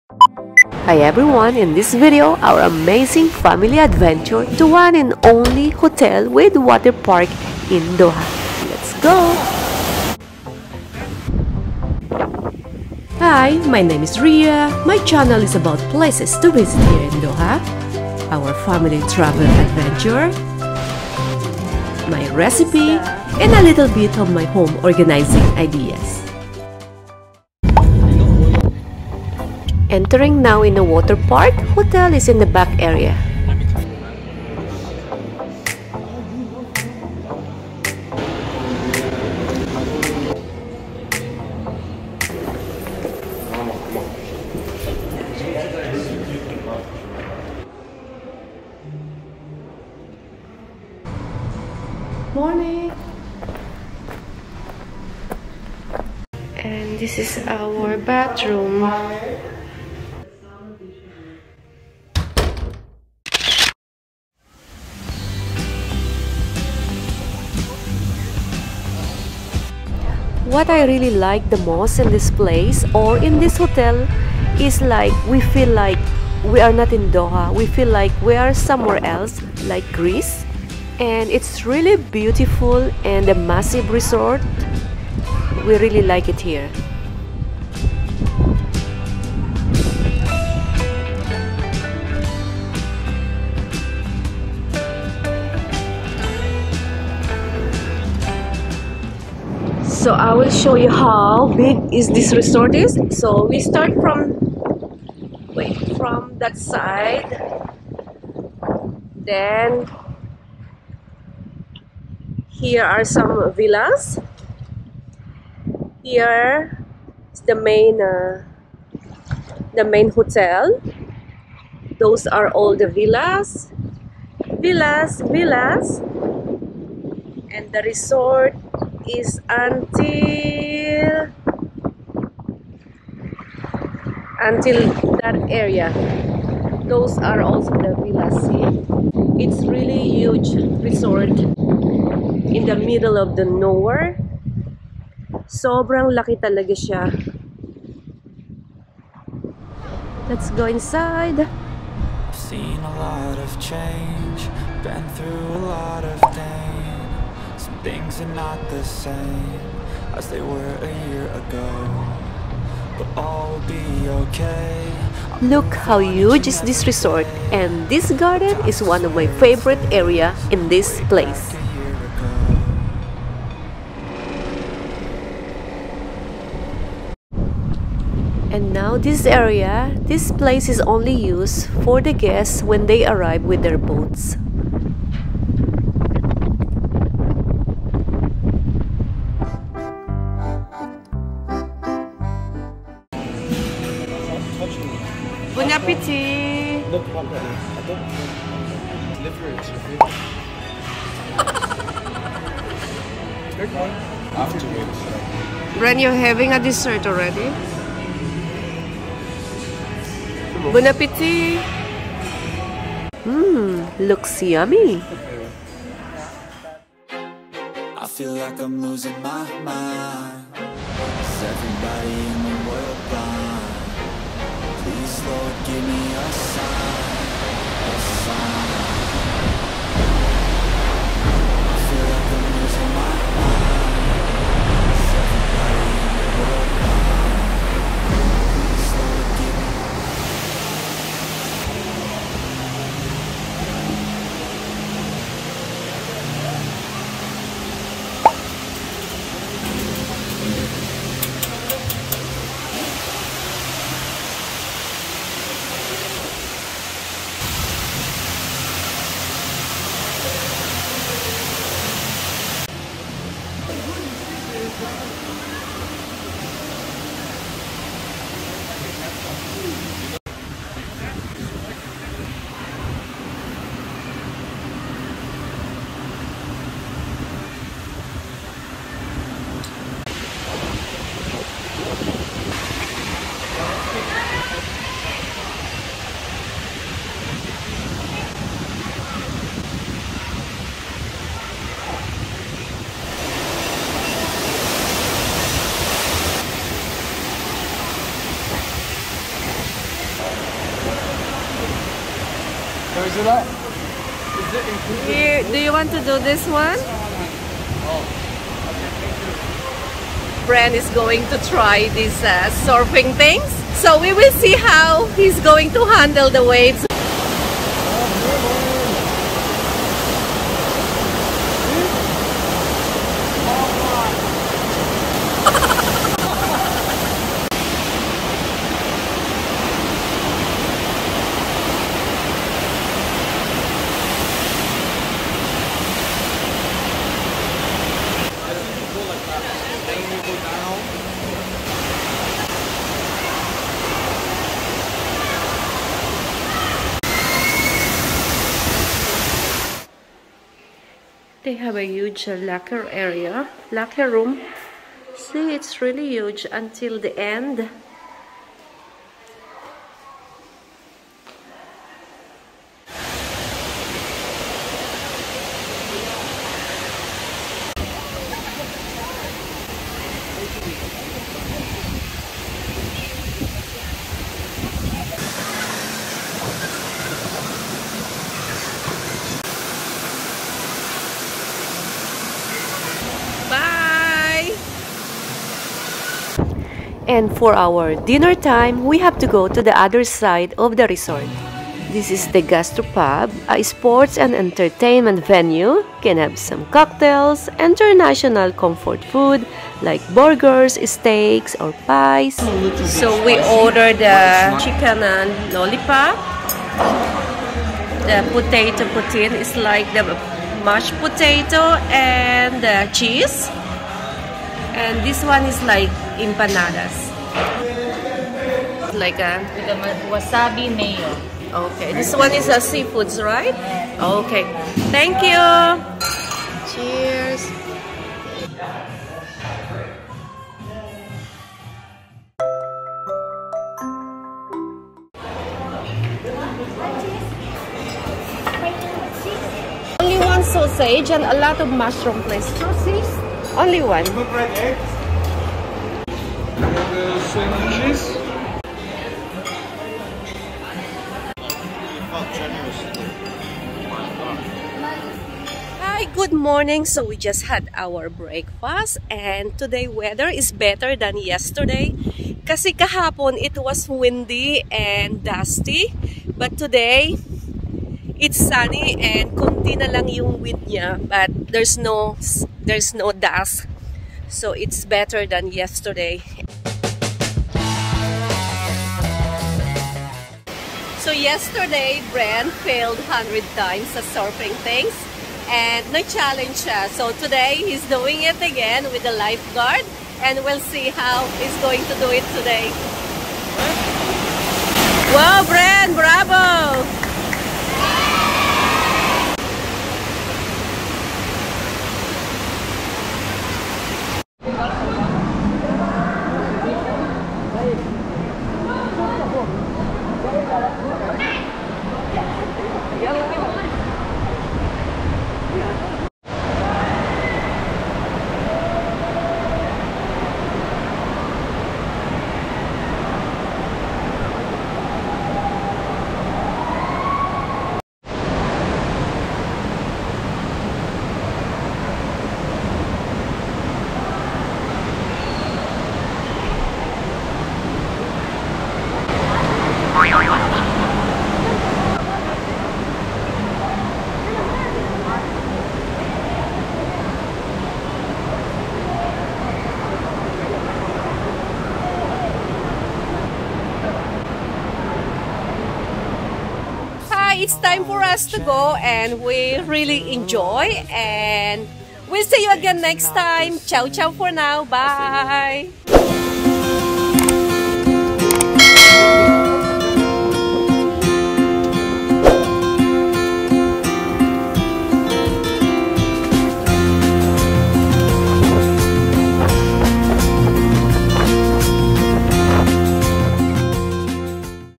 Take a Peek Inside Hilton Salwa Beach Resort Doha / Full Tour Inside
Our Amazing adventure at Hilton Resort Salwa the one and Only hotel with Water Park in the middle of the dessert.

Ria Soss